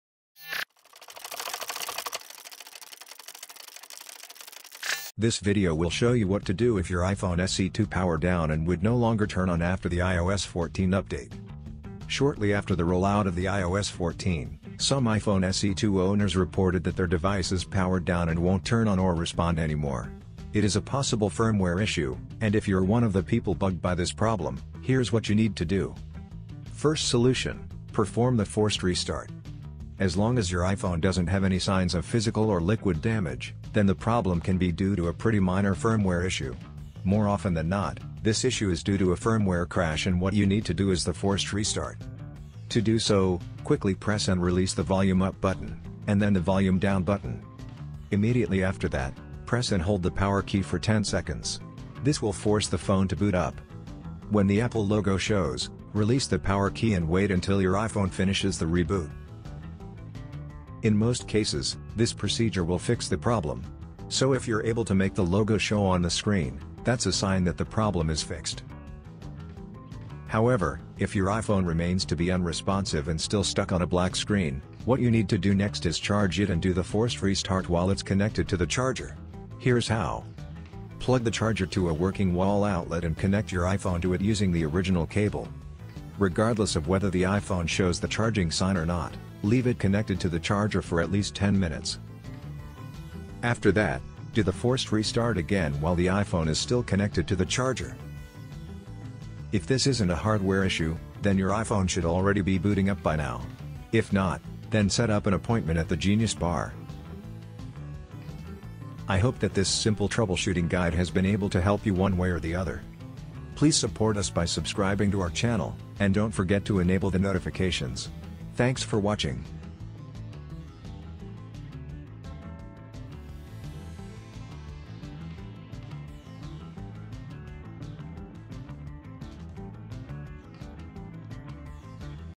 How To Fix It When iPhone SE 2 Won’t Turn On After iOS 14
This video will show you what to do if your iPhone SE 2 powered down and would no longer turn on after the iOS 14 update.

Shortly after the roll out of the iOS 14, some iPhone SE 2 owners reported that their devices powered down and won’t turn on or respond anymore. It is a possible firmware issue, and if you’re one of the people bugged by this problem, here’s what you need to do:

Timecodes
00:00 - Introduction
00:26 - Perform the Forced Restart
01:14 - Charge your iPhone SE 2 and do the Forced Restart